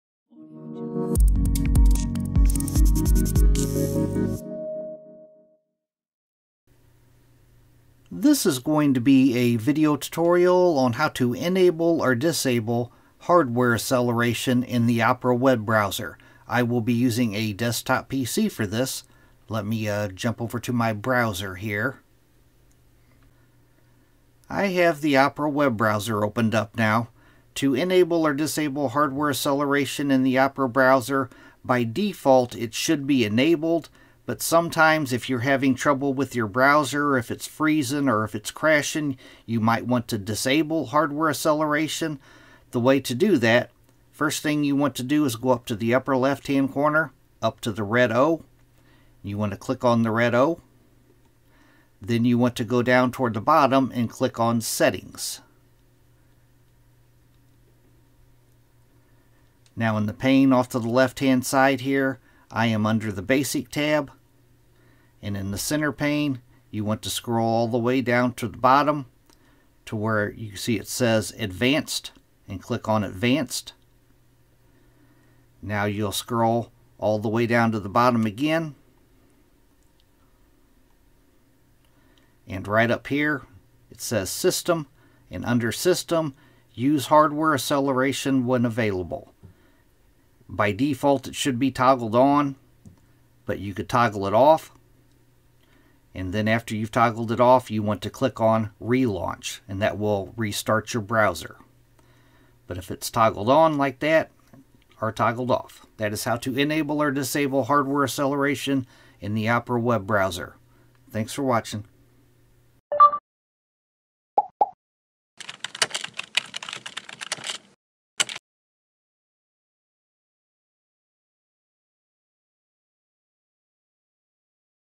How To Enable Or Disable Graphics / Hardware Acceleration On The Opera Web Browser | PC Tutorial 👍
In this tutorial, learn how to enable or disable graphics acceleration in the Opera browser on your PC. Enabling graphics acceleration can improve performance, especially for graphic-intensive websites, while disabling it can help troubleshoot display issues.

Simple Steps
1. Open the Opera web browser
2. Click on the "O" in the upper left hand corner.
3. Click on "Settings" from the dropdown.
4. Select "Browser" from the left side pane.
5. Scroll down to the "System" area and toggle on or off "Graphics Acceleration".

Chapters
0:00 How To Turn On Or Off Graphics Acceleration In The Opera Browser
0:19 Open The Opera Browser
0:29 Click On The O In The Upper Left Corner
0:39 Select Settings
0:52 Select The Browser Tab
1:10 In The Center Tab Scroll And Toggle On Or Off JavaScript